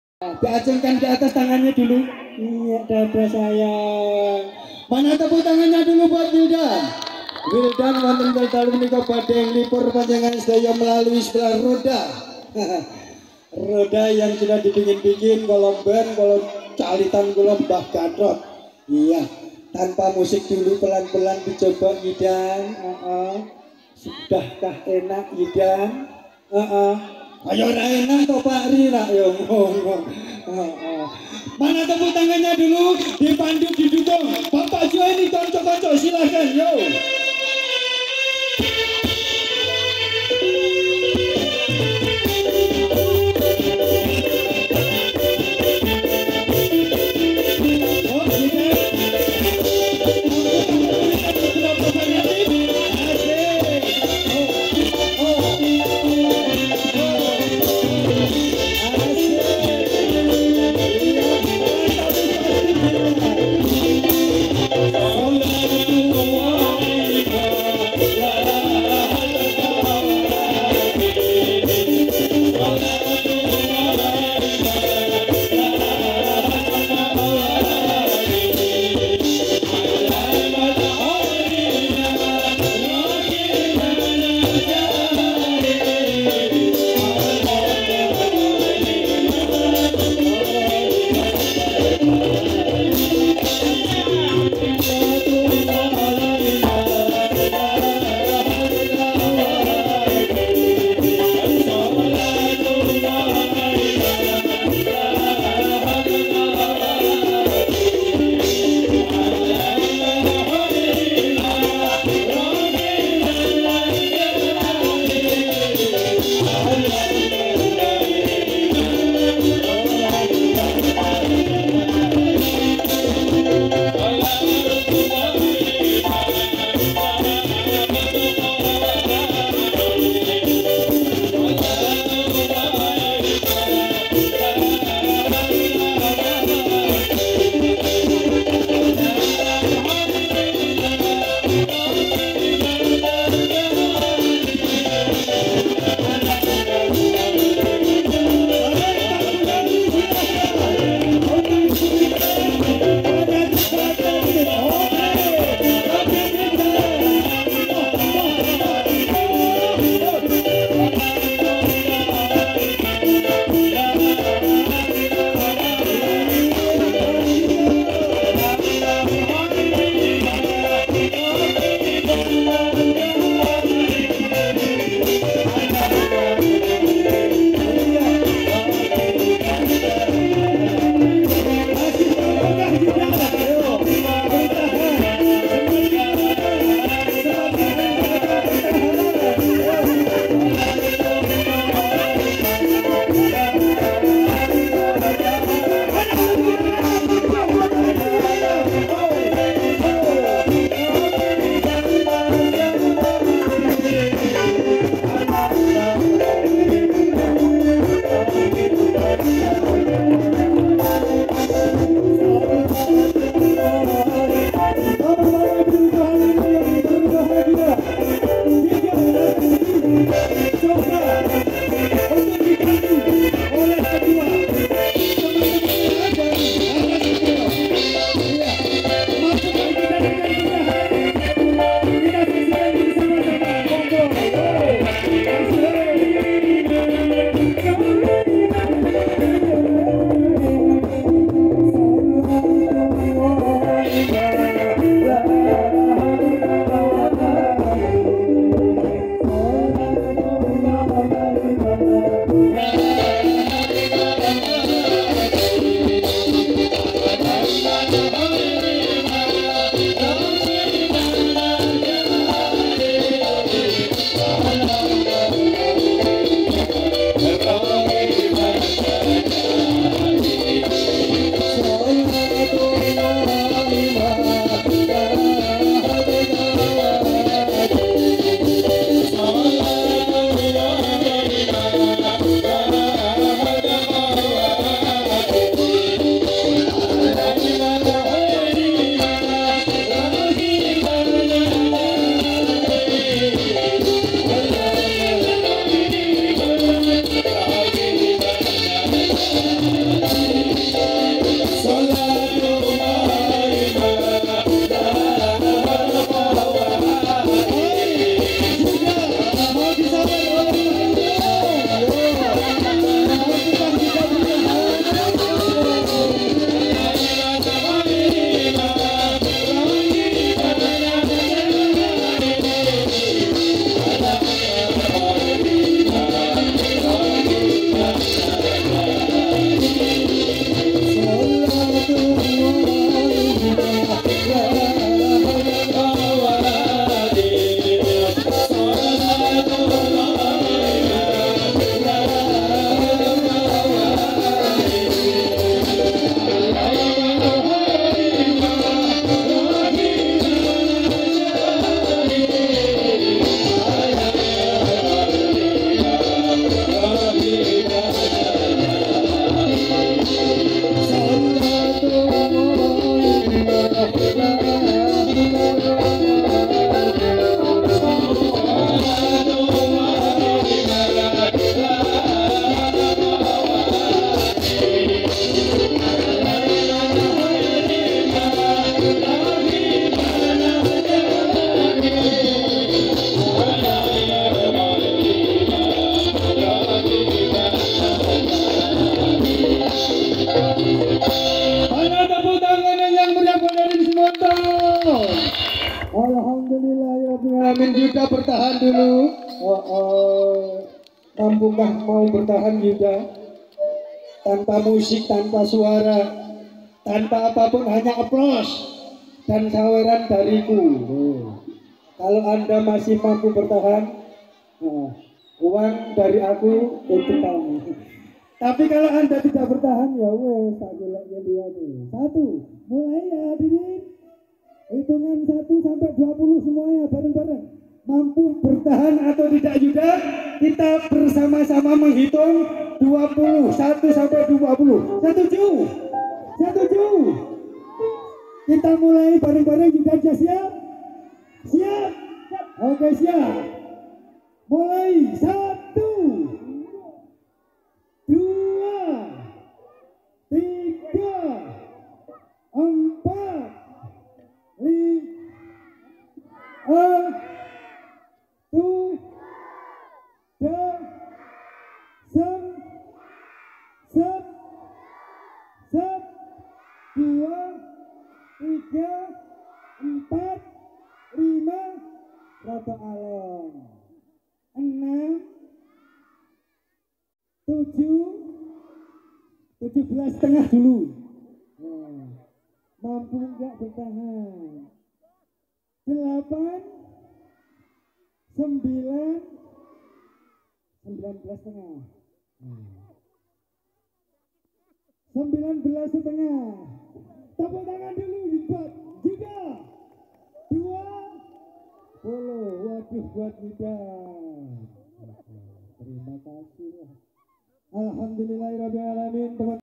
atraksi berdiri diatas roda dapat saweran bersama Badui Laras muda gantalan
saat tampil dilapangan minomartani Badui Laras muda kubro Siswo gantalan minomartani saat sesi atraksi berdiri diatas roda,ada yang memberi saweran